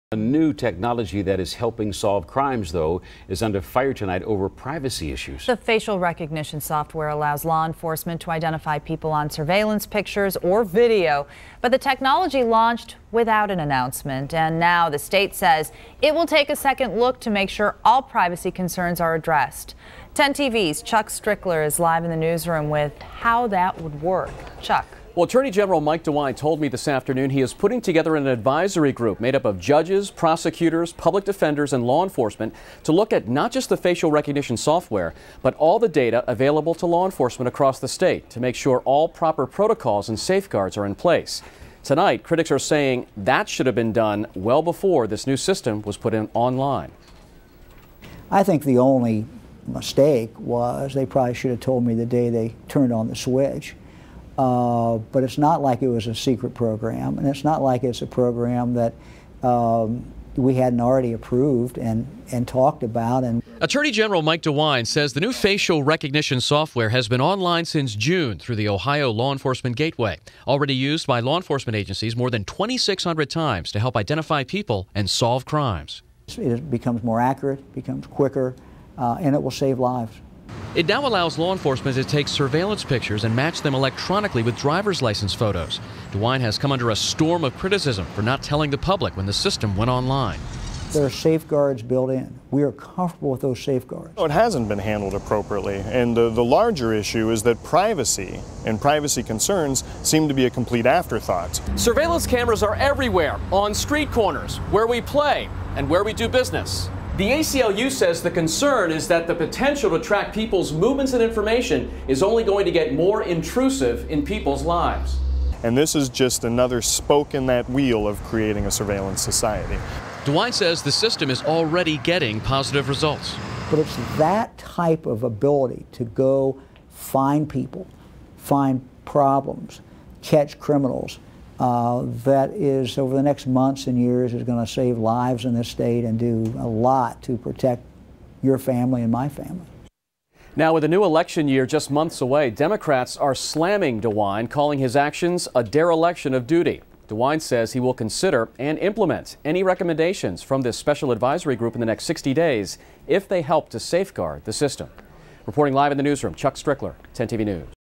Facial Recognition Questions
The ACLU raises concerns over a facial recognition software.